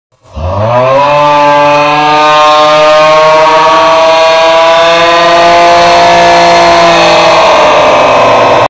2011 Toyota Premio aughhh meme
lol, btw it sounds like Lamborghini Aventador instead of snoring